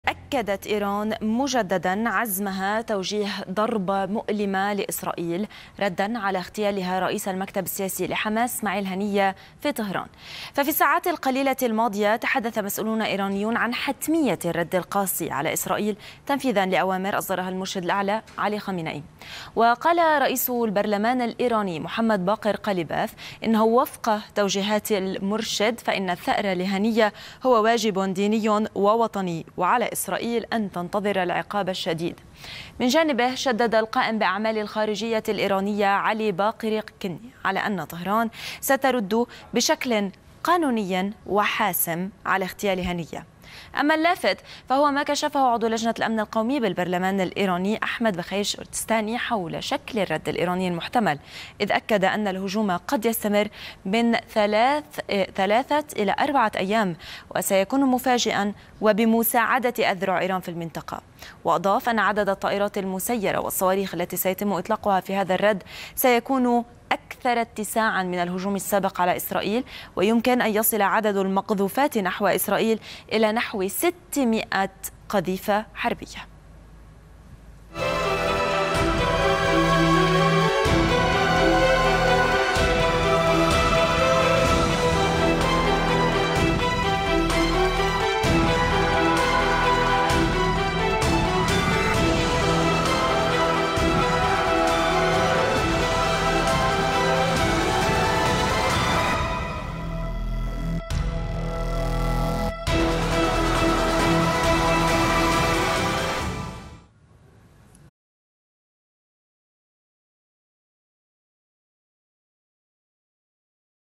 إيران تعلن مجدداً عزمها توجيه "ضربة قاسية" لإسرائيل
تنفيذاً لأوامر خامنئي بحتمية الرد على اغتيال إسماعيل_هنية.. مسؤولون إيرانيون يتحدثون مجدداً عن عزم طهران توجيه "ضربة قاسية" لـ